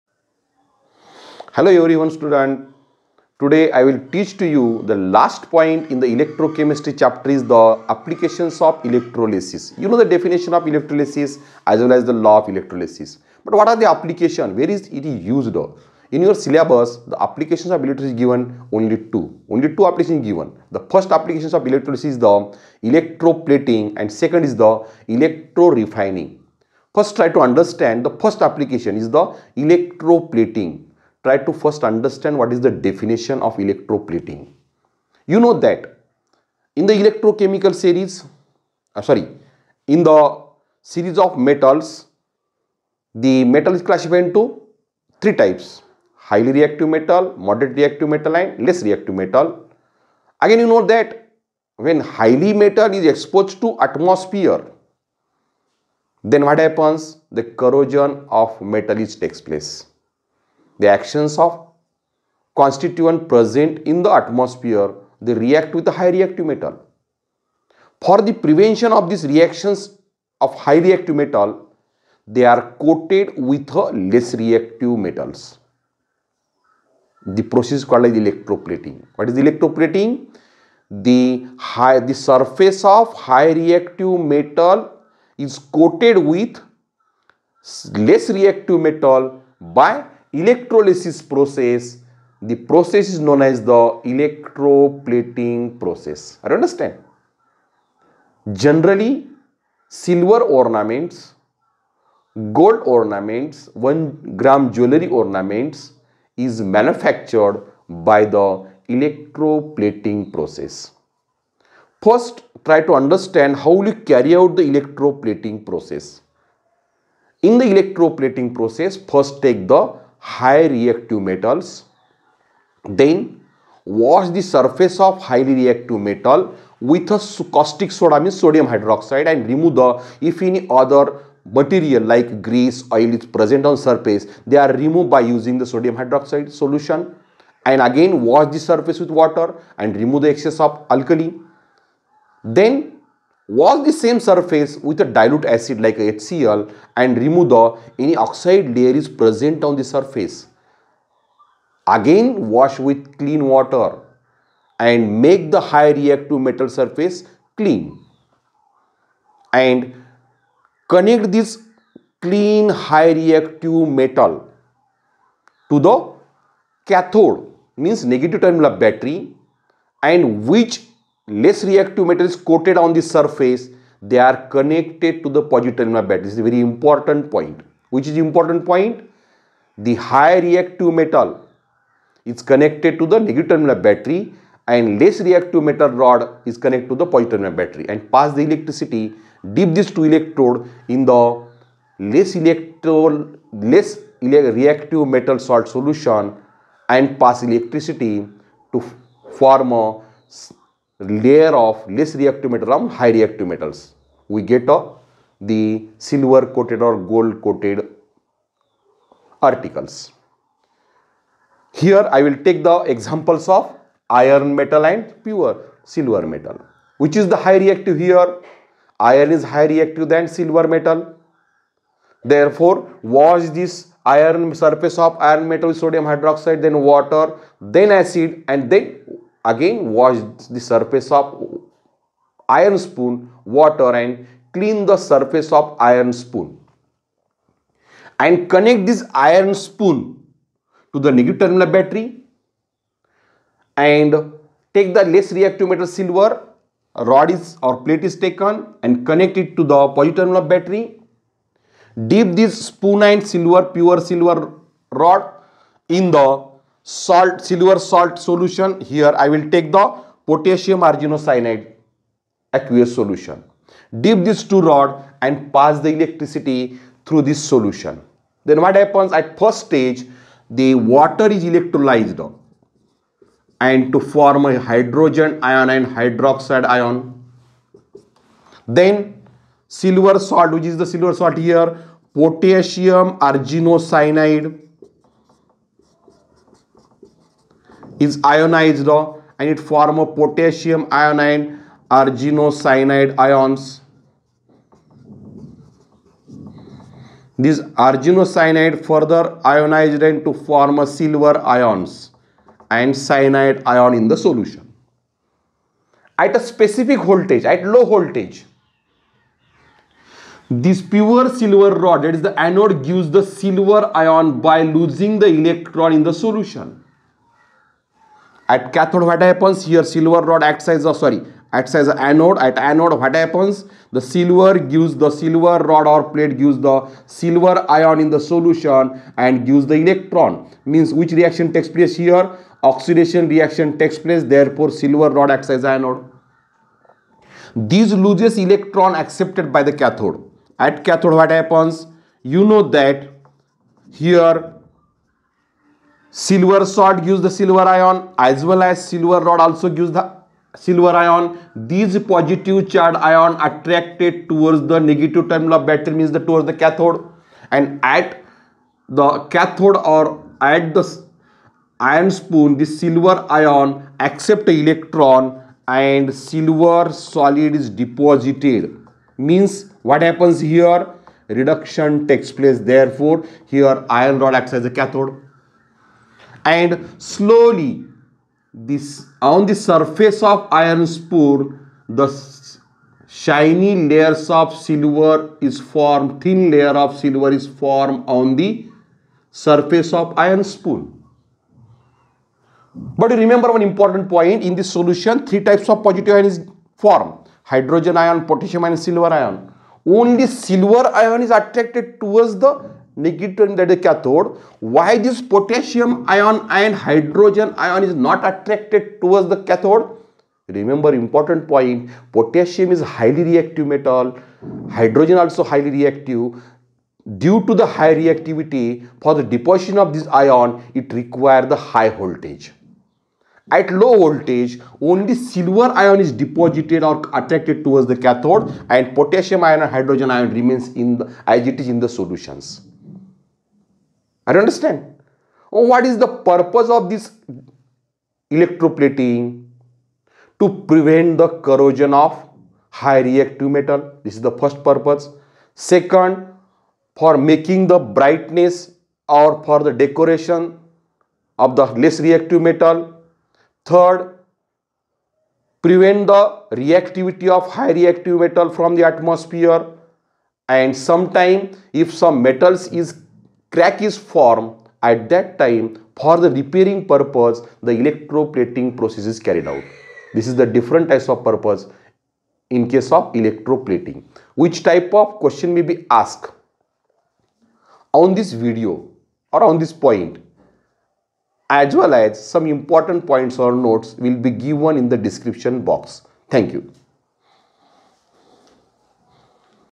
Electroplating |Application of electrolysis |electrochemistry |polytechnice1st y.|MSBTE| Diploma F.Y
Electroplating  :- The process in which less reactive metal coated with highly reactive metal by electrolysis process is called electroplating.e.g. Silver plated spoon , Gold plated ornaments .
 Purpose :- Electroplating is carried out for following purpose :-
1) Protection from corrosion
2) Decoration:- A coating of superior metal like gold , silver, nickel etc. over the inferior base metal is applied in order to increase beautiful and atrractive appearance.
3) Repairing :- The broken pieces or worn out machine parts can be repaired by electro deposition of metal in between the broken parts of metal .
Electroplating of silver on iron spoon :- 1) A plating of silver on iron spoon is done in a rectangular tank made up of steel. 2) Iron spoon is first thoroughly cleaned by boiling with caustic soda to remove grease and dirt.3) Then it is washed with water until free from caustic soda . Then it is treated with dil. HCl to remove any oxide layer. Again it is washed with water and polished carefully.4) The iron spoon is then made as cathode. The pure plate of silver metal whose coating is desired is made as anode. The cathode and anode both are immersed in cell of potassium argento cyanide. 5) When current is passed ,the iron spoon  gets plated with a smooth and bright deposit of silver. the silver anode gets slowly dissolved in solution by giving Ag+ ion with releasing electrons.
6) The silver ions from the potassium argent cyanide and which are formed by dissolution of anode get discharged at the cathode and silver gets deposited on the iron spoon. 7) The more active K+ and H+ ions require very high voltage for their discharge so they remain as it is in the form of ions in the solution.
At anode :- Ag Ag+ + e-
At cathode:- Ag+ + e- Ag (s)
Homework :-
Q1) Write short notes on :- Electroplating
Q.2) Multiple choice questions:-
i) In electroplating process less reactive metal rod or plate acts.
  a) cathode    b) anode    c) Both a and b   d) None of these
ii)When silver is plated on iron spoon then the cathode is made up of .
 a) iron spoon   b) silver   c) carbon   d) platinum
iii)The process of obtaining metallic coating on an article is known as.
  a) electro refining     b) electrometallurgy  c) electrotyping  d) electroplating
iv) In electroplating process for removing oil or grease the surface of metal is washed with .
  a) dil. Hcl     b) caustic soda  c) water    d) None of these
v) Oxide layer formed on the surface of metal is removed by washing with .
  a) water  b) caustic soda   c) dil HCl    d) None of these